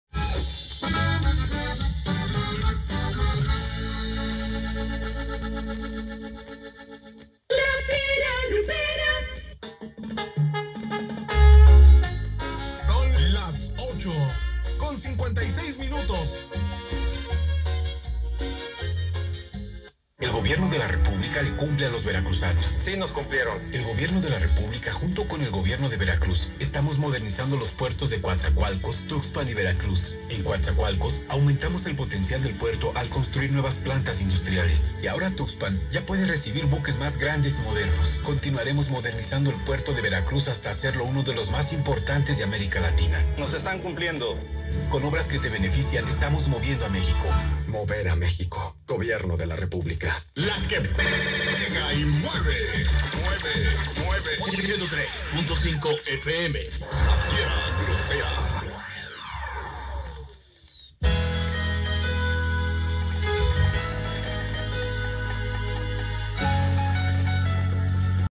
FM DX TROPO XHPV-FM 103.5 MHz Papantla, Ver. captada en Reynosa, Tam. 22/12/2014 (629 Km)
La Fiera Grupera 103.5 MHz de Papantla, Veracruz captada en Reynosa, Tamaulipas el 22 de diciembre de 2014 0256 UTC - 629 Km (390 mi)